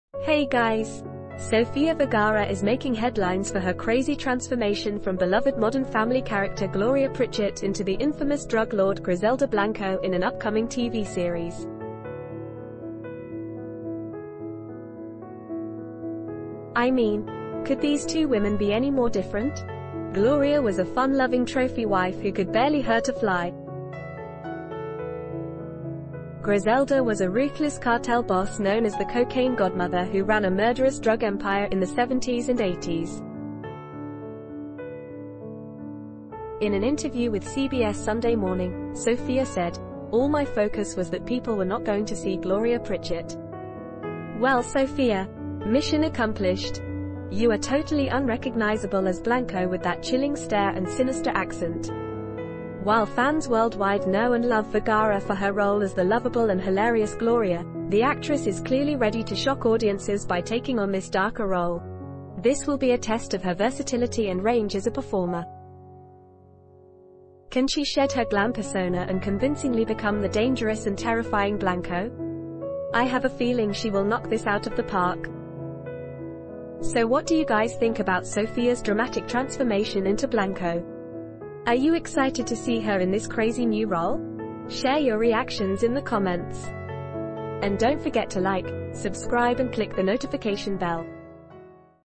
Gloria to Griselda: Sofia Vergara's Dramatic Turn as Narco Trafficker
Sofia Vergara is making headlines for her crazy transformation from beloved Modern Family character Gloria Pritchett into the infamous drug lord Griselda Blanco in an upcoming TV series.

So what do you guys think about Sofia’s dramatic transformation into Blanco? Are you excited to see her in this crazy new role? Share your reactions in the comments!